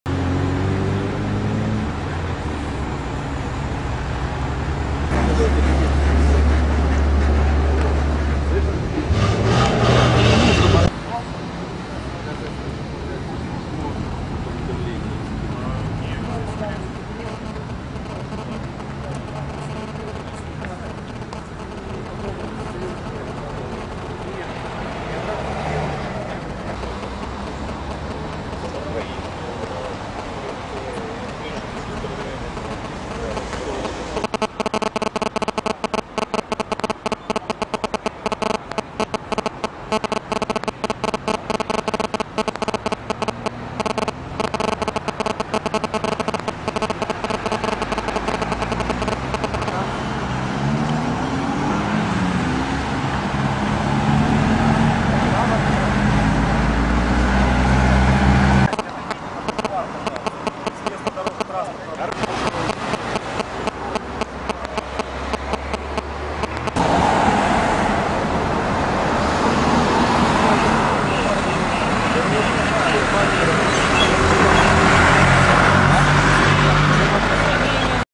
Кровавое ДТП на Трассе Севастополь-Ялта
На трассе Севастополь - Ялта произошло страшное ДТП.
На нерегулируемом перекрестке столкнулись рейсовый автобус, автомобиль Ford Mondeo и микроавтобус Ford Transit.
По предварительным данным, водитель рейсового автобуса маршрута «Севастополь-Ялта» во время левого поворота, не пропустил легковой «Форд».
После первого удара Ford Mondeo  перевернулся, задел микроавтобус и вылетел далеко на обочину в виде бесформенных обломков. Во время катастрофы на месте погиб 23-летний водитель Ford Mondeo.
Его 31-летняя пассажирка c тяжелыми травмами  попала в больницу. Также пострадала 60-летняя пассажирка рейсового автобуса. В пенсионерки диагностировали многочисленные ушибы и гематомы.
По факту смертельного ДТП открытое уголовное дело.